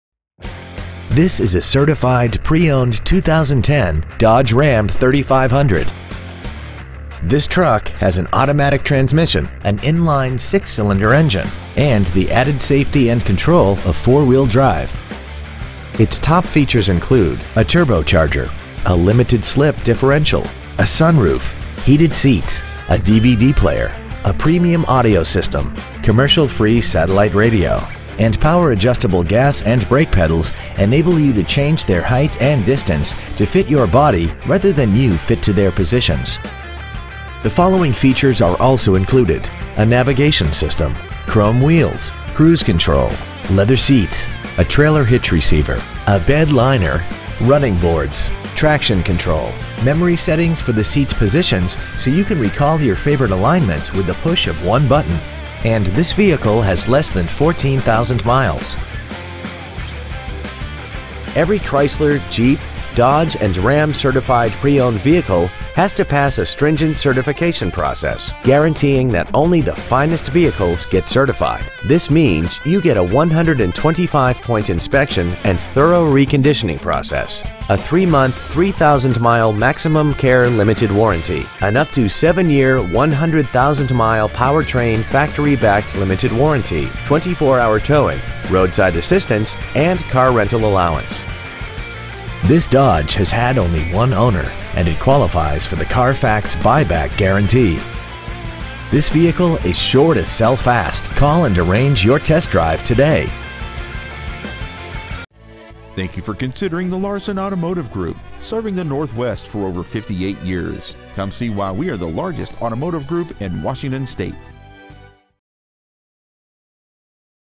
2010 Dodge Ram 3500 Puyallup WA
Year : 2010
 Make : Dodge
 Model : Ram 3500
 Engine : 6.7L I6 Turbo Diesel
 Trans . : Automatic
 Exterior : Light Graystone Pearl
 Miles : 13,696
 Interior : Dark Slate
 Stock : 9342

 2010 Dodge Ram 3500 Puyallup WA